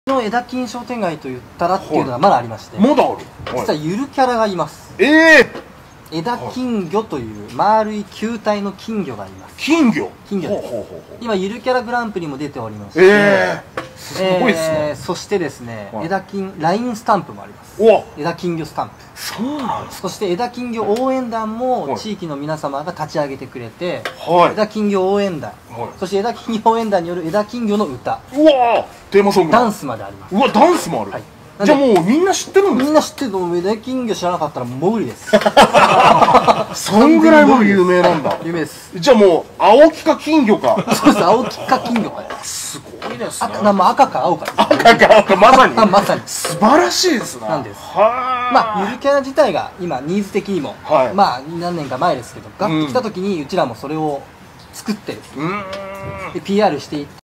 横浜市都筑区えだきん商店会の魅力を会長に聞きました！
【ハピゴラTV】
横浜の商店街を紹介するロケ番組！

今回は都筑区はえだきん商店会みやはら会長に商店会の魅力をお聞きしました！